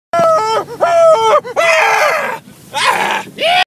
Germans in Norway sees moose for the first time
Moose in Norway